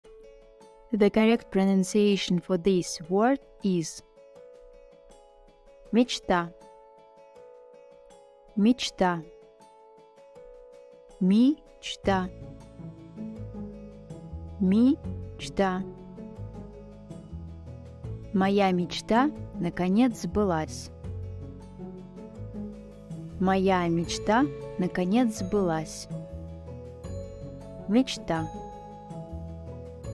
How To Pronounce Красный (100% NATIVE Speaker!!)
Want to learn how to pronounce "Красный" like a native speaker?  Our pronunciation guide will take you through the pronunciation step - by - step, making sure you get it right every time.